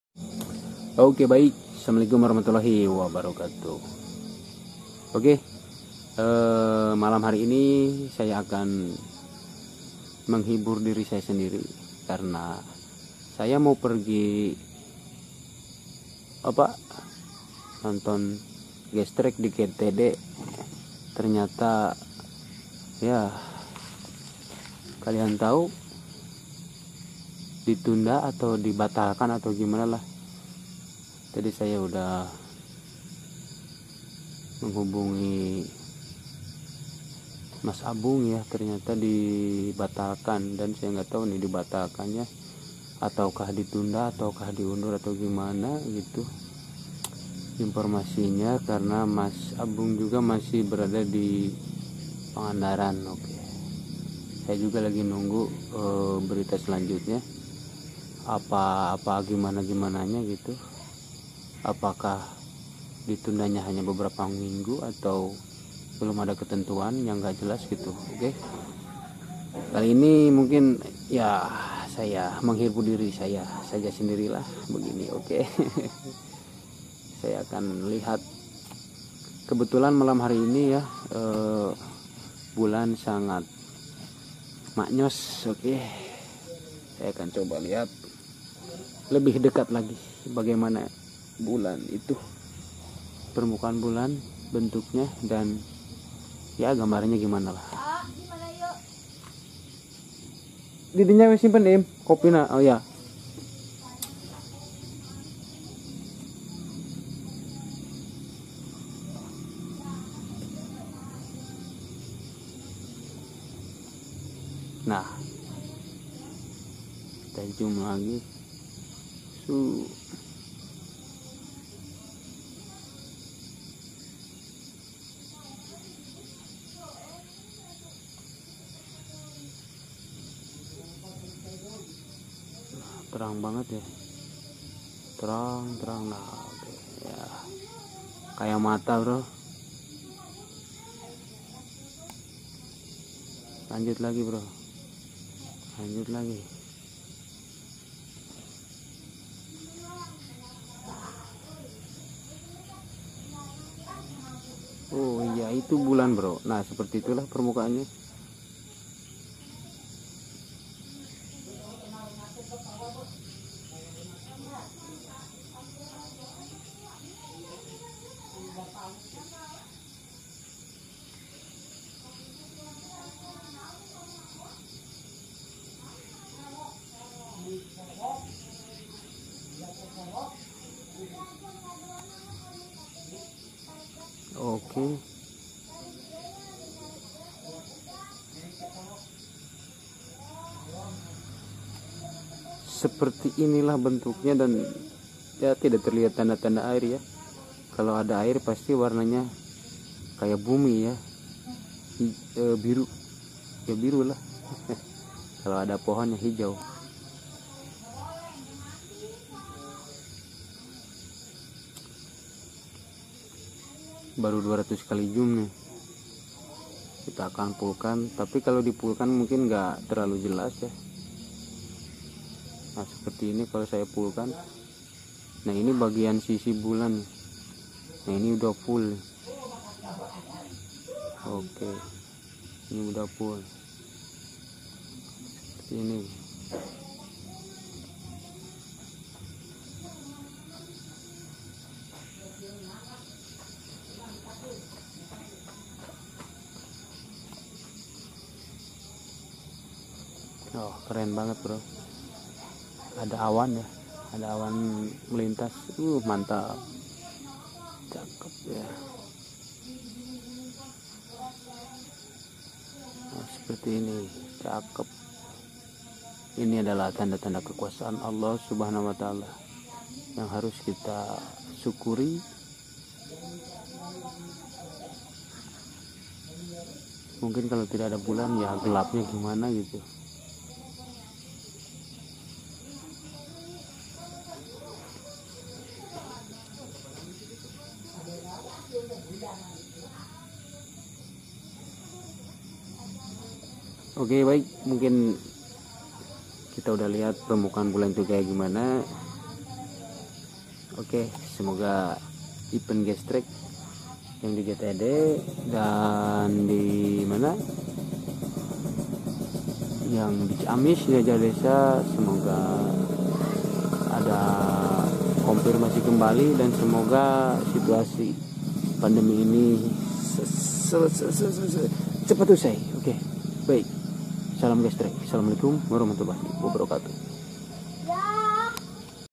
GTD CUP?....GRASSTRACK YANG TERHAMBAT KE 2 kalinya...efex nya malah?.....
grasstrack
gtd cup 2020
tertunda ke 2 kalinya. pastinya panitia,pembalap,penonton dan media liput sangat kecewa,tapi disisi lain kita harus patuhi peraturan protokol pemerintah...untuk mengobati hati ini,aku shoot bulan ajja. hehehehe. semoga seluruh insan otomotif tetap semangat .